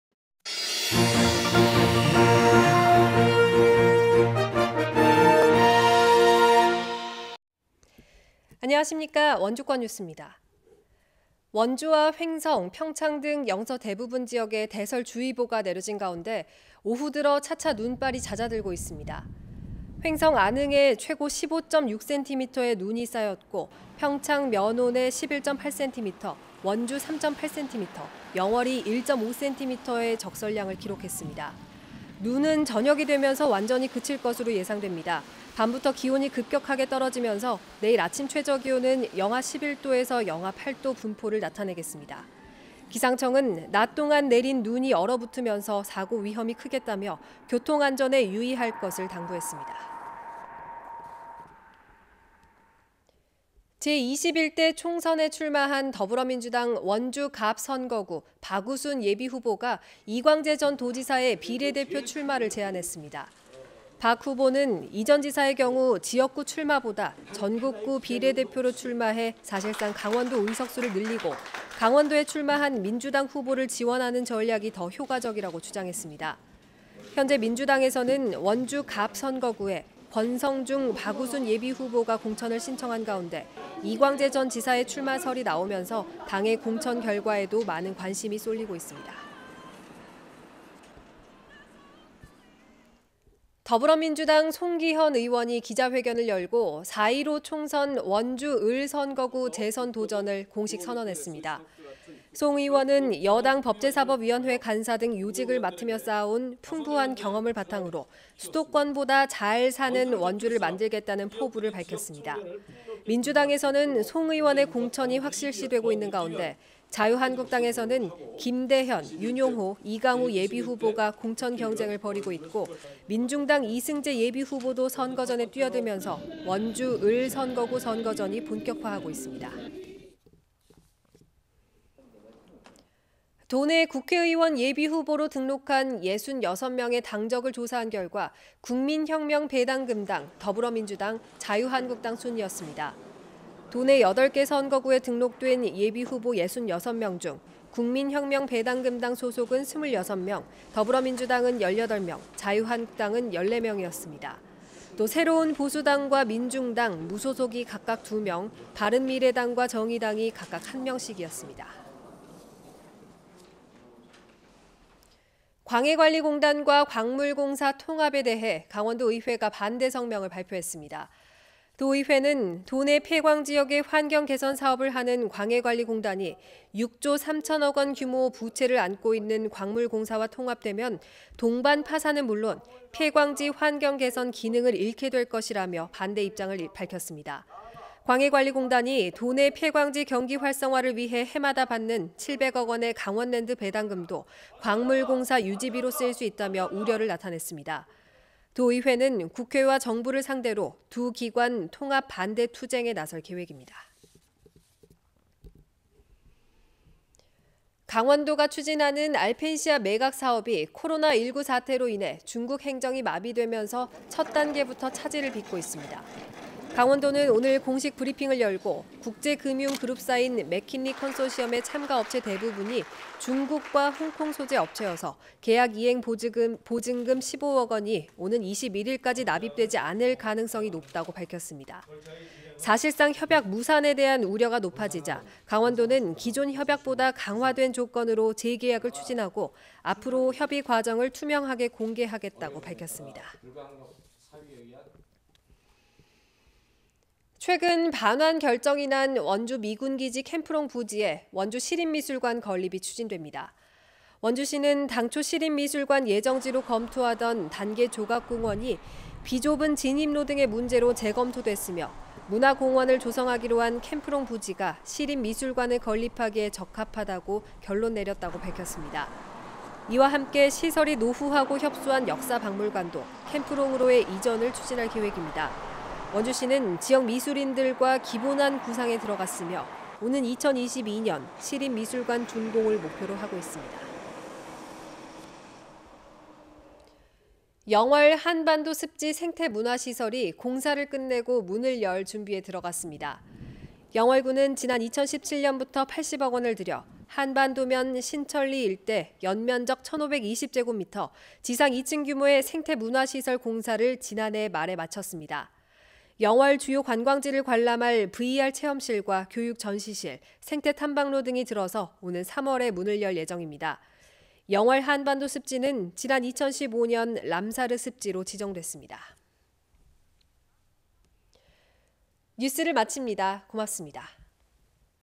2020. 2. 17 5NEWS
- 영서 대설주의보.. 횡성 안흥 15.6cm
- 박우순, 이광재 비례대표 출마 제안
- 민주당 송기헌, 원주을 재선 도전 공식선언
- 총선 예비후보 66명, 정당은 배당금당 소속 많아
- 도의회, 광해관리공단-광물공사 통합 반대 성명
- 알펜시아 매각 첫단계부터 '삐걱'
- 원주)시립미술관, 캠프롱 부지에 건립 추진
- 영월)한반도습지 생태문화시설 준공